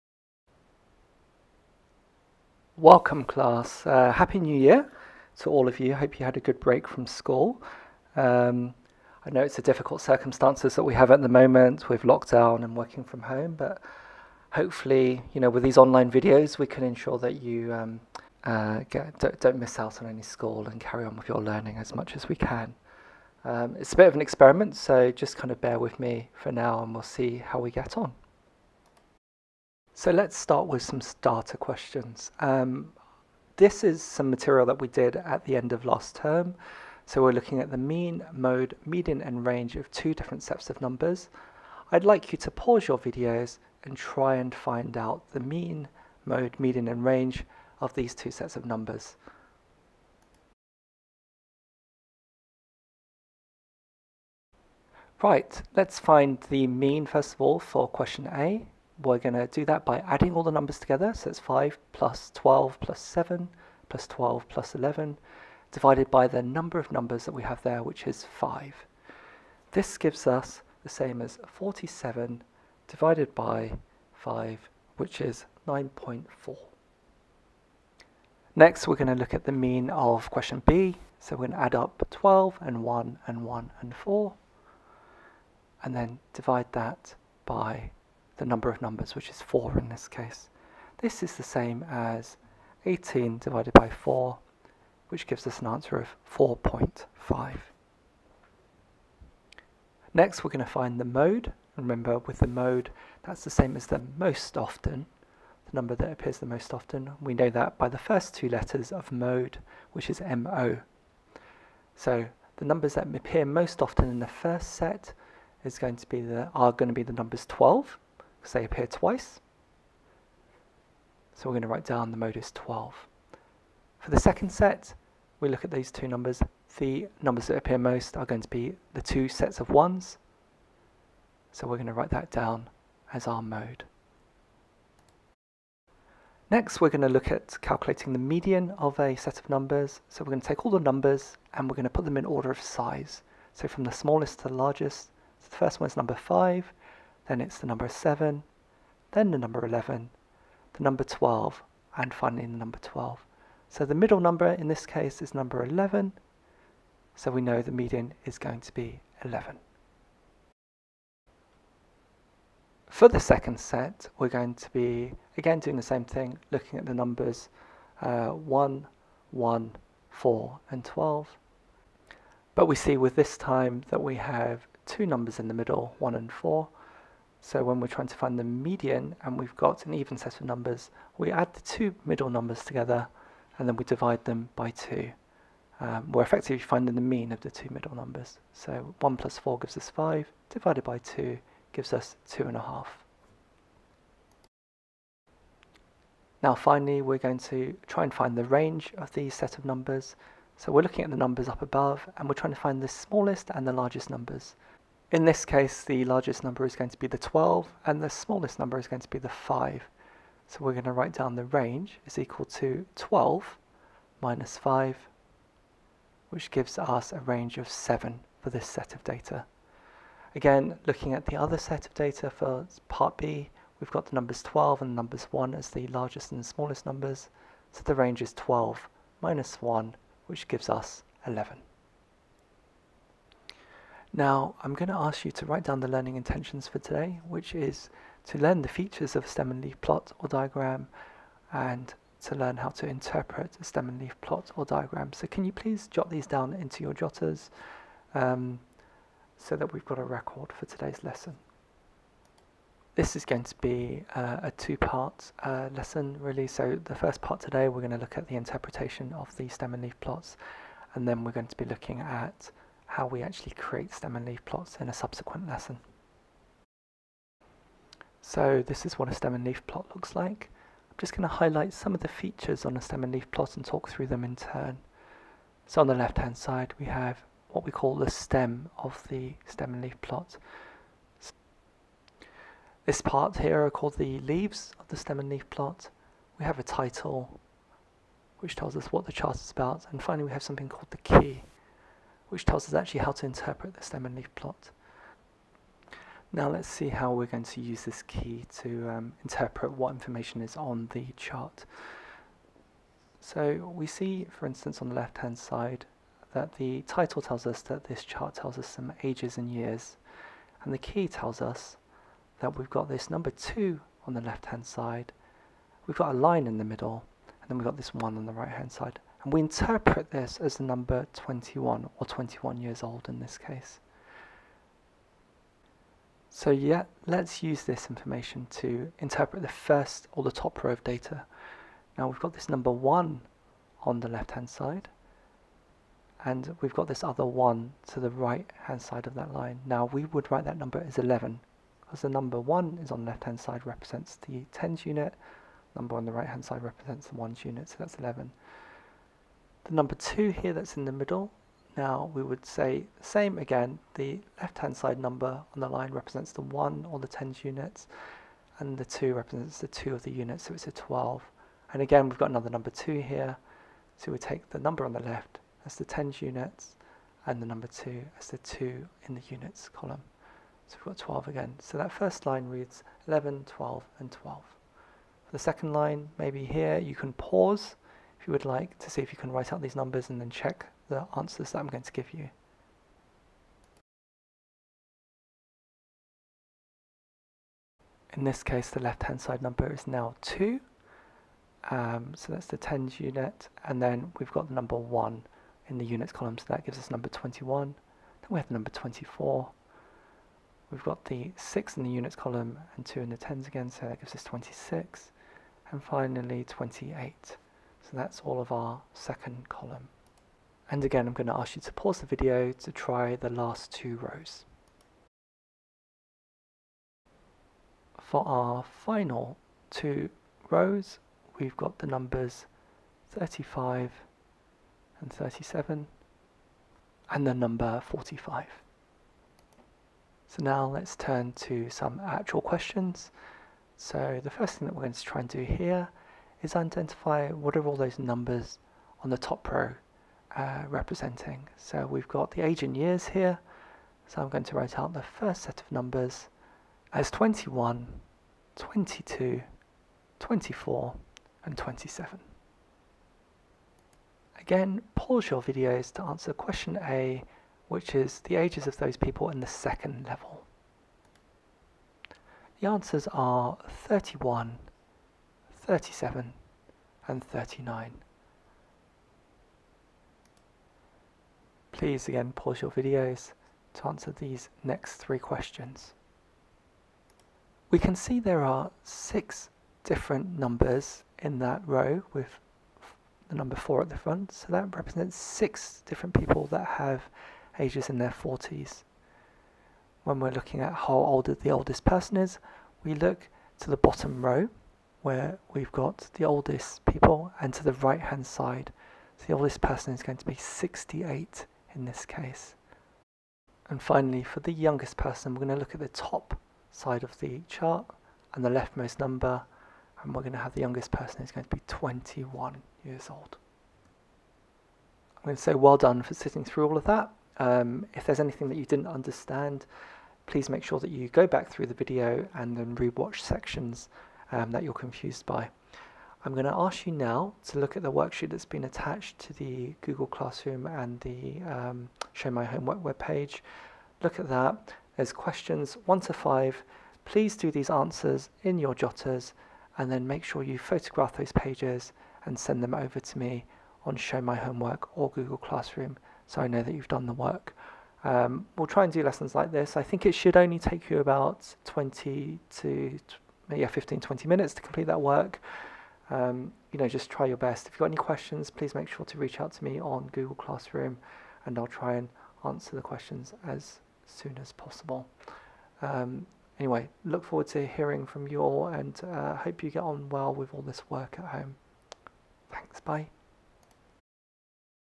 2CM1 - Lesson #1: Interpreting Stem and Leaf Plots
Lesson outlining how we interpret Stem and Leaf Plots